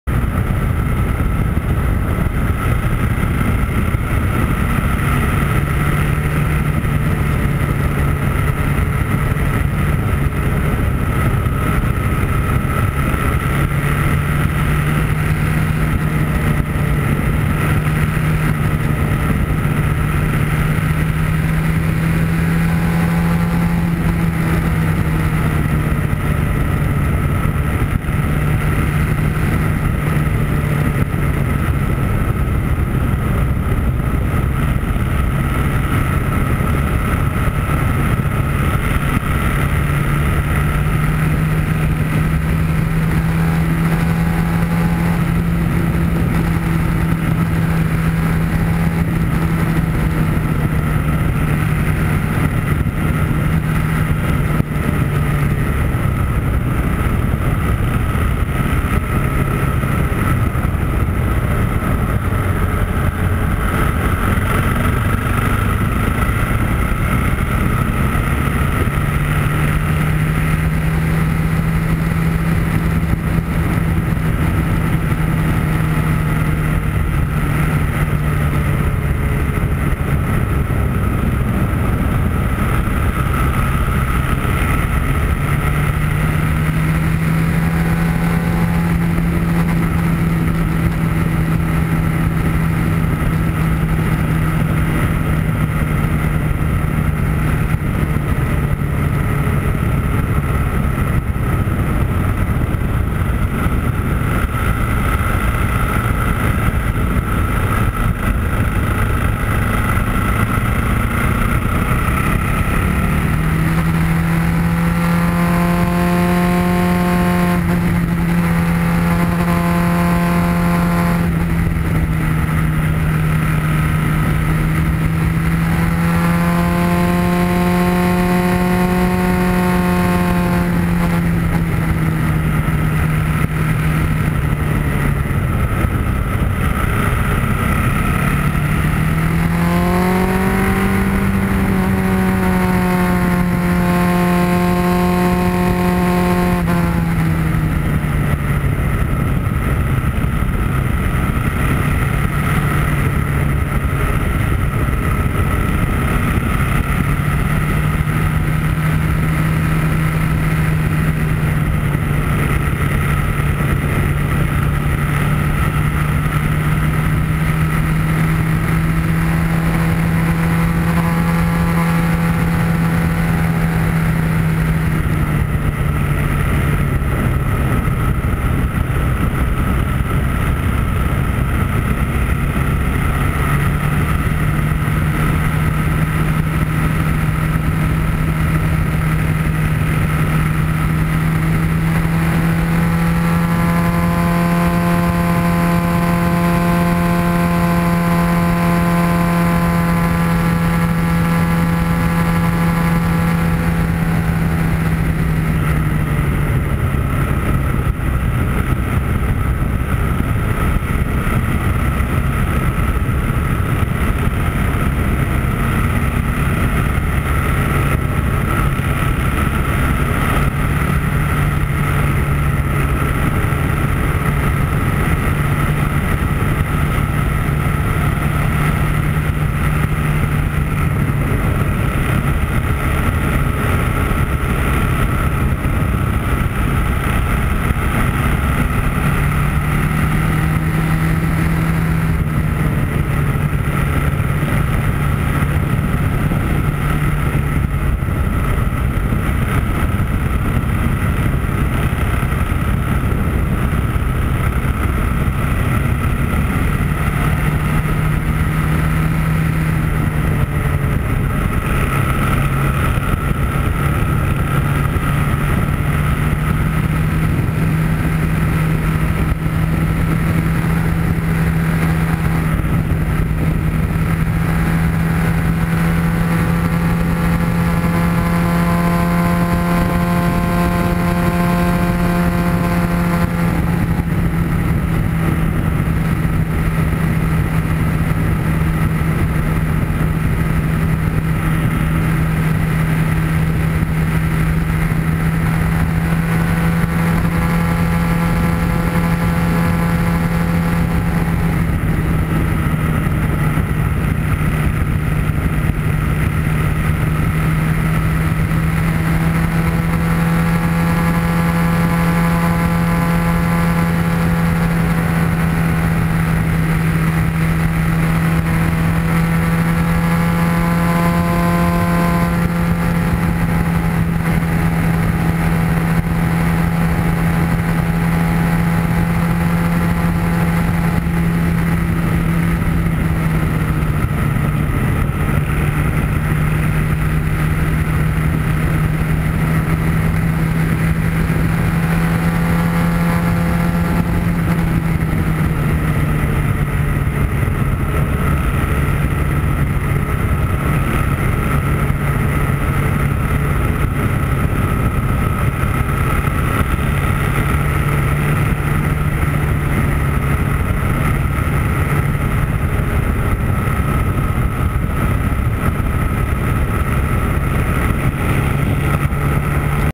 Highway 1 North - Big Sur - KTM Duke 690
Northbound on Highway 1, around Big Sur in California

On a 2014 KTM Duke 690